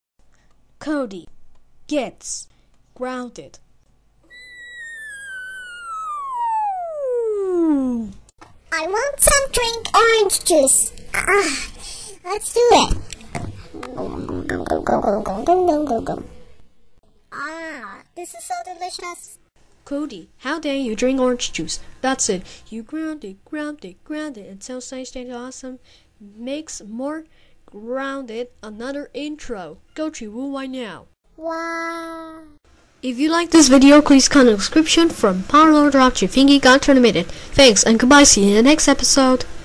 Cody Eats Orange Juice And Gets Grounded (REMAKE)
I Hate My Old Cody Gets Grounded Intro

I'm Also Get Cody Gets Grounded Intro 3 Later